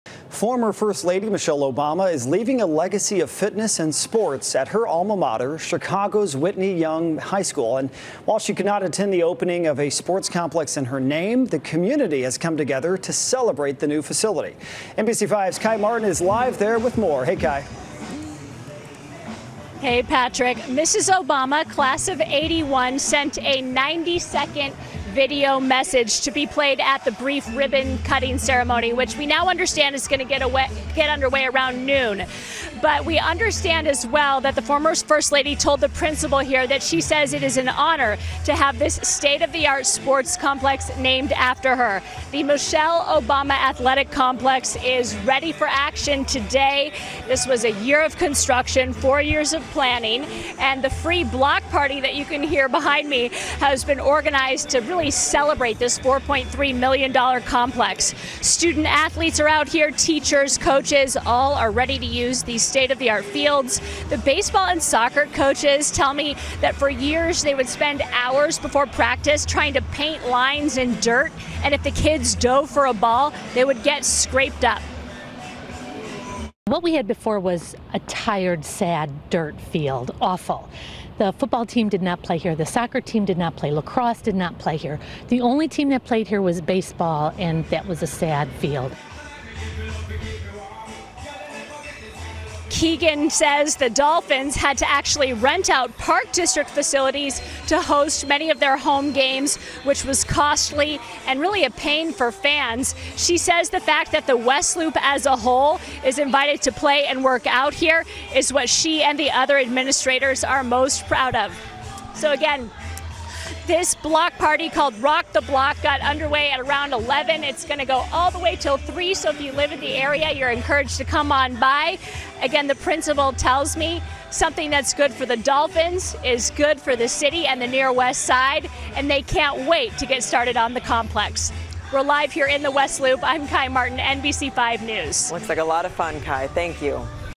New Athletic Field dedicated to Michelle Obama | NBC Chicago
Whitney Young Magnet High School honored former first lady and Chicago native Michelle Obama on Monday with a new athletic complex in her name. Kye Martin reports.